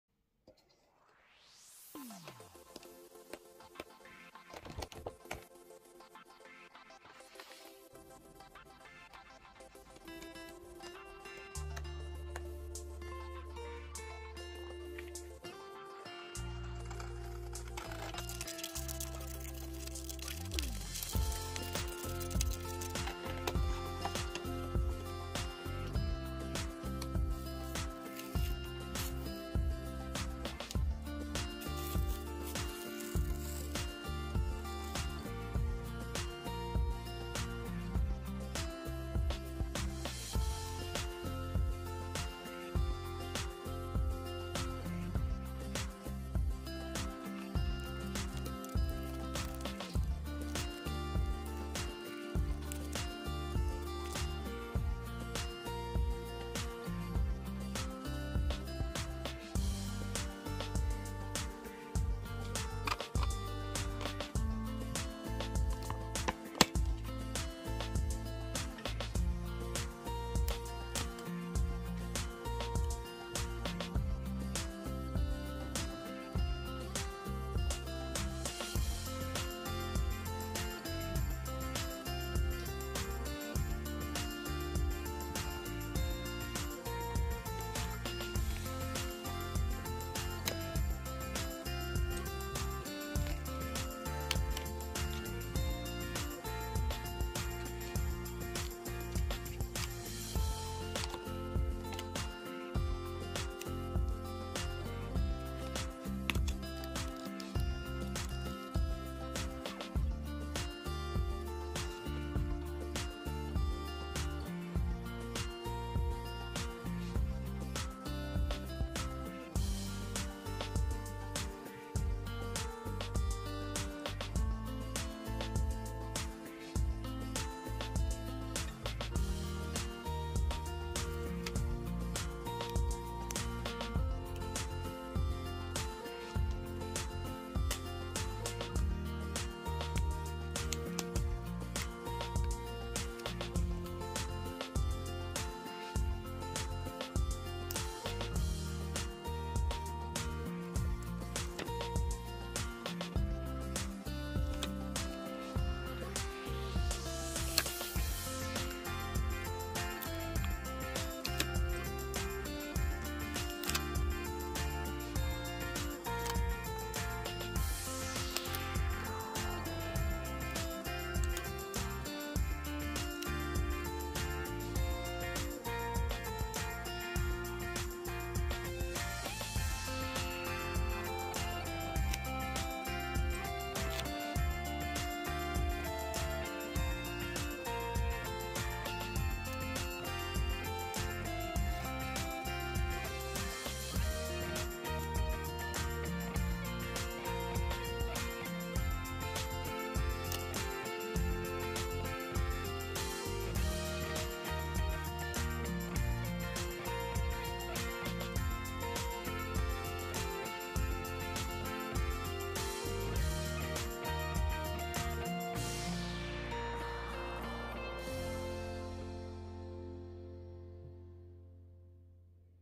Be One With Nature Scrapbook
As you lot know I love nothing more than get outside into the Wonderful Outdoors and interacting with Wildlife Especially Horses